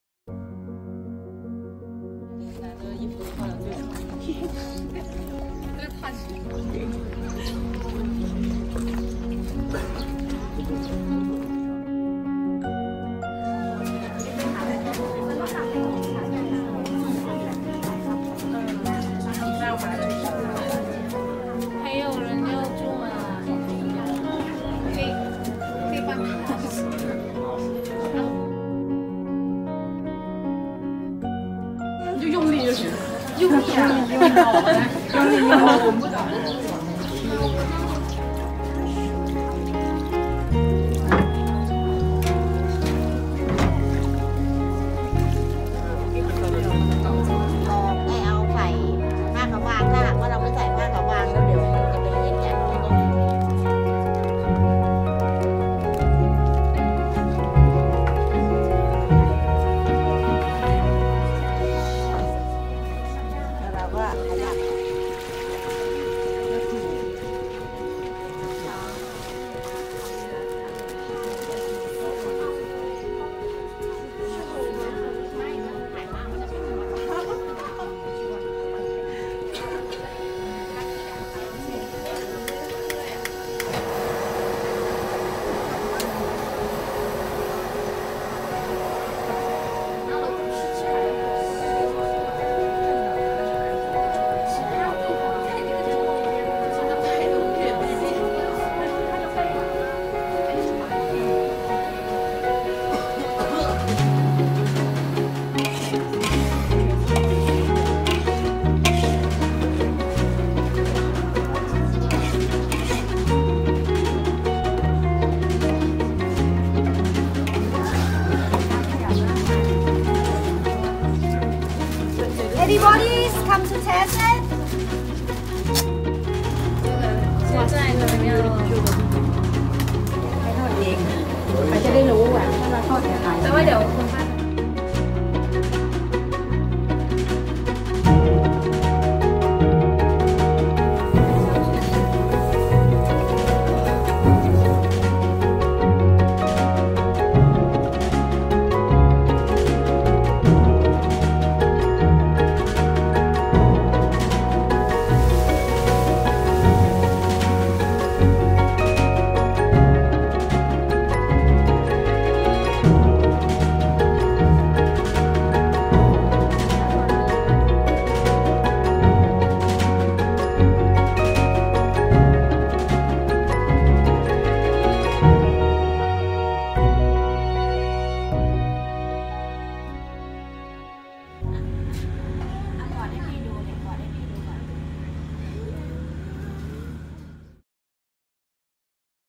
The CSR campaign in Ayutthaya (บ้านเกาะเก็ด)
This is an event organized by Rangsit University's Mass media department , Ayutthaya trip. The site of the event is  บ้านเกาะเก็ด（baan goh ged）, a small resort near Ayutthaya, where you can experience the primitive customs of central Thailand.